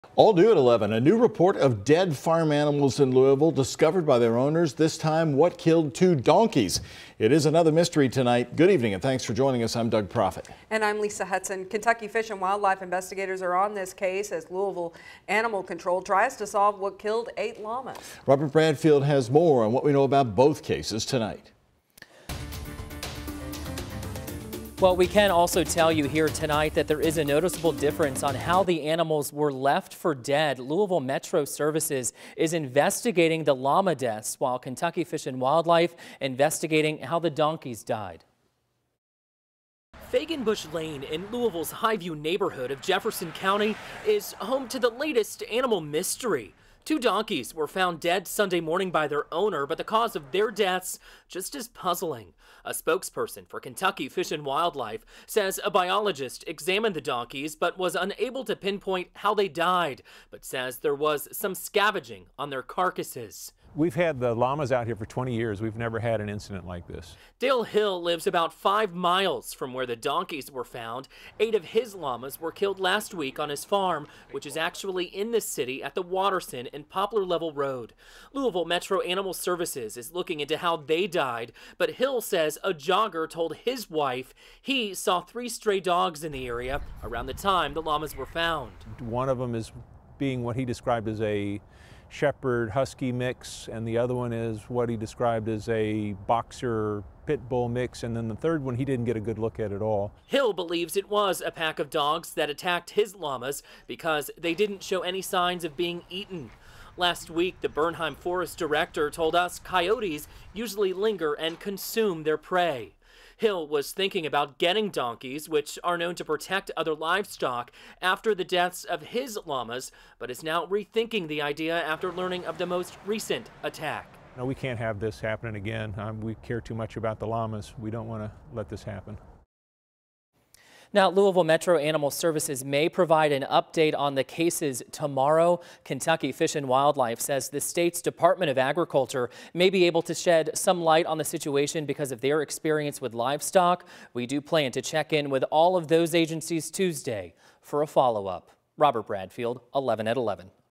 Donkeys killed near recent llama attack
Another mystery plagues the area after two donkeys were killed not too far from a recent llama attack.